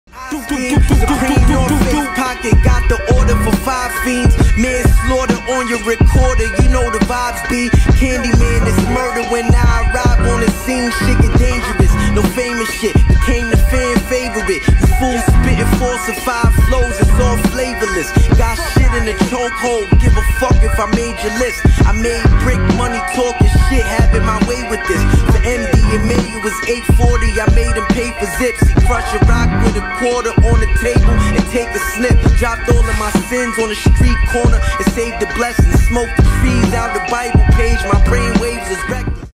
Rome Streetz live in Miami 5/7/22 Recap Video
@RomeStreetz1 @WestsideGunn @GriseldaRecords  Rome Streetz concert Recap Video May 7, 2022 at The Joint of Miami

Shot by Lion Muhammad